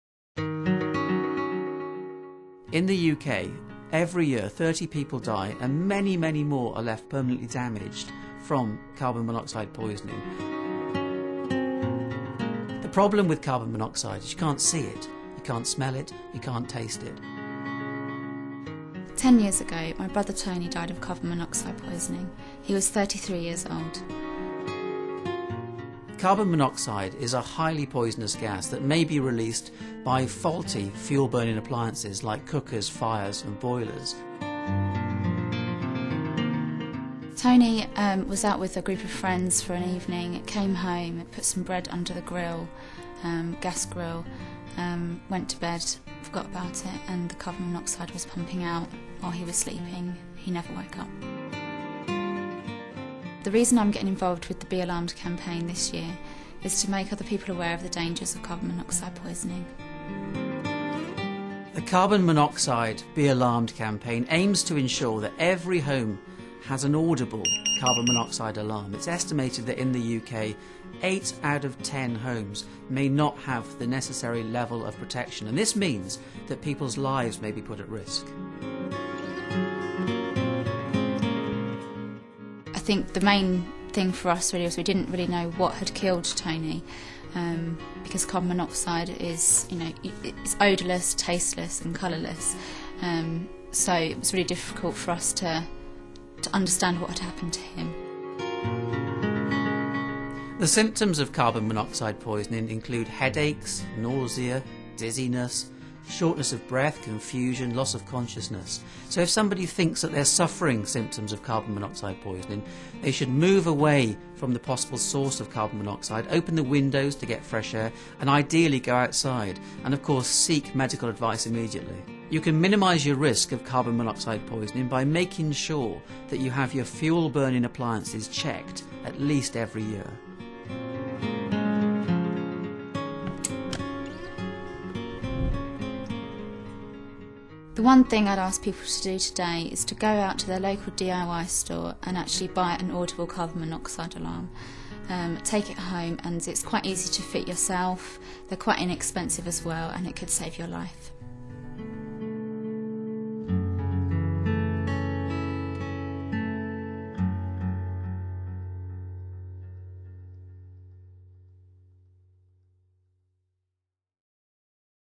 Dangers of Carbon Monoxide
Carbon Monoxide is produced when fuel-burning appliances, such as boilers and cookers are incorrectly fitted, badly repaired or poorly maintained. In this video Melissa Matthews, whose brother Tony died of Carbon Monoxide poisoning and Dr Rob Hicks, explain the dangers, the symptoms and just how easy it can be to make your home safe.